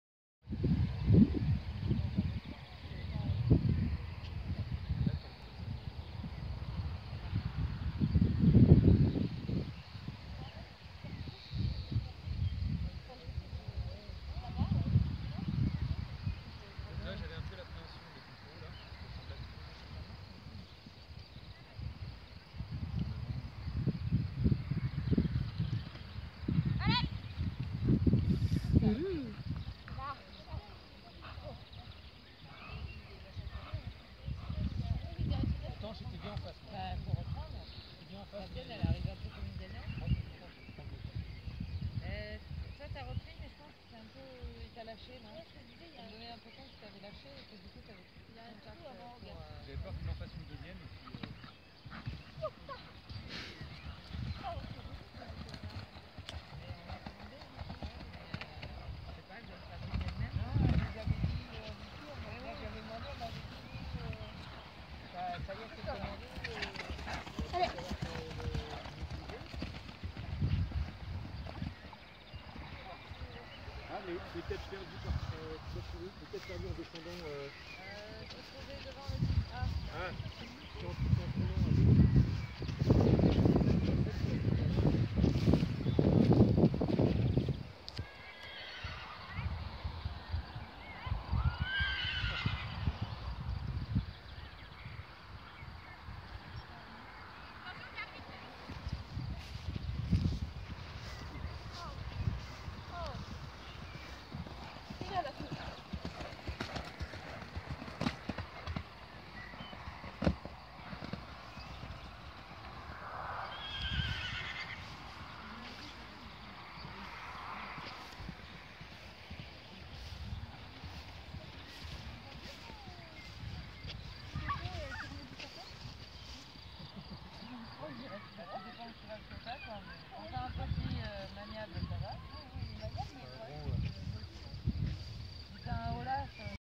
Margaux & Nice CCE club 2 Villers en ouche le 12/06/2016 : CROSS
Apres l’élimination sur le cso on attend la fin des épreuves pour aller sur le cross et faire notre parcours club 2 normal